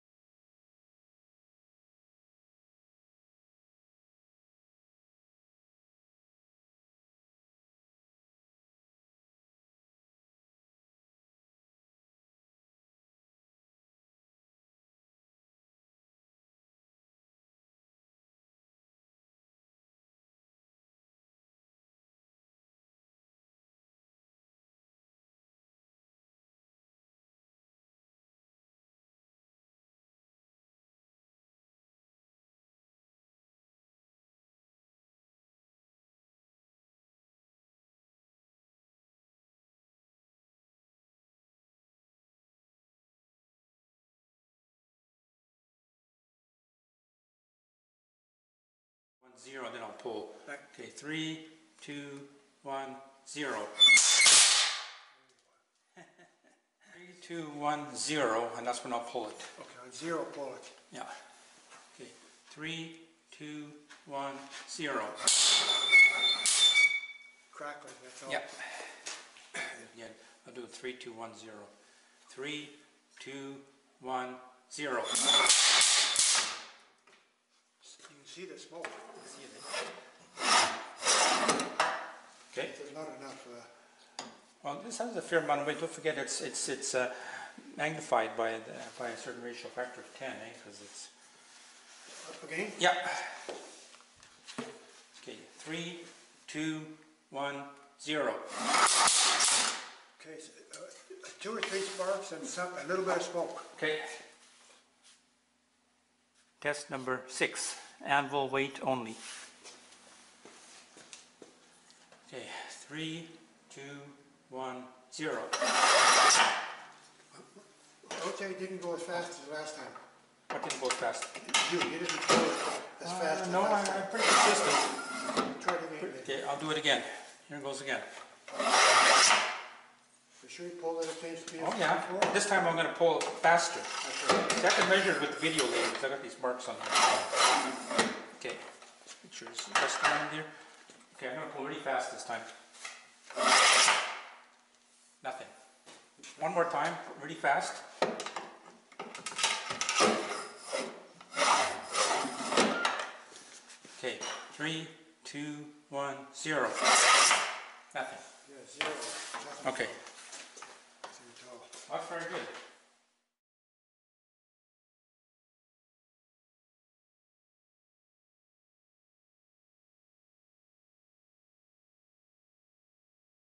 Rocket Propellant friction testing series A
This is the initial trial of an apparatus built to test the friction sensitivity of rocket propellants. Initial testing is solely to see how well the device works, and uses a special highly friction-sensitive specimen (not a rocket propellant).